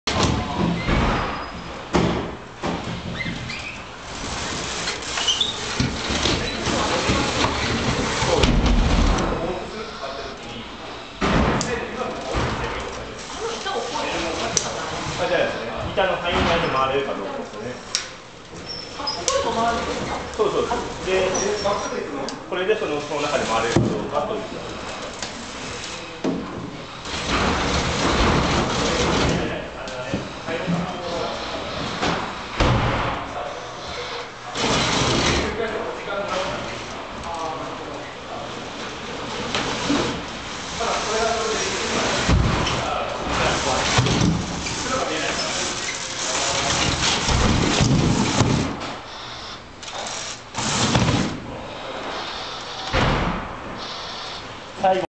UMRS2009、STAIRS(35°）.AVI
昇降できた最大角度の傾斜を10回連続で計測し、平均時間を記録する。